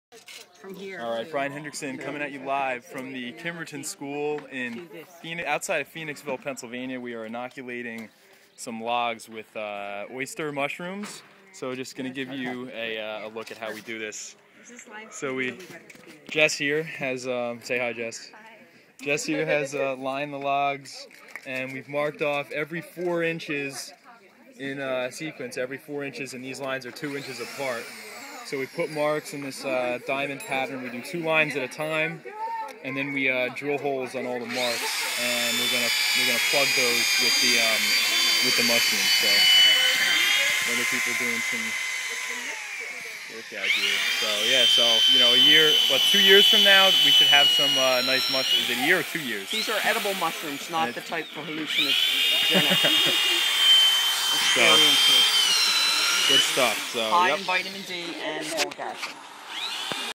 Permaculture Workshop: Oyster Mushroom Log Innoculation
Drilling holes in logs to innoculate them with shitake and oister mushrooms.  Workshop took place at the Kimberton School outsidenof Phoenixville, PA